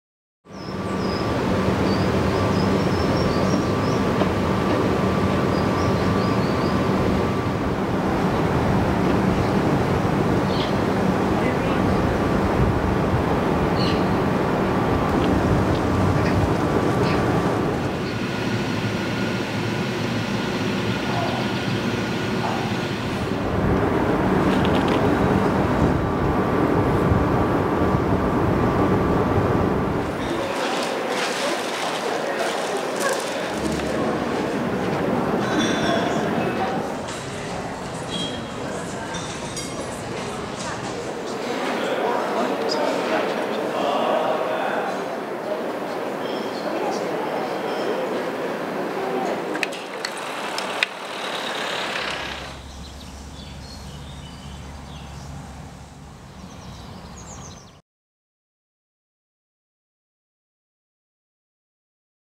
Sainsbury Centre for Visual Arts by Foster + Partners
Sainsbury Centre for Visual Arts
Norwich, UK 1974 – 1978

The Sainsbury Centre integrates a number of related activities within a single, light-filled space. Structural and service elements are contained within the double-layer walls and roof. Within this shell is space that incorporates galleries, a reception, the Faculty of Fine Art, a common room and a restaurant. The main gallery evokes the spirit of the collection’s originally domestic setting. The new wing extends the building below ground level, exploiting the contours of the site to emerge in the form of a glazed crescent incised in the landscape.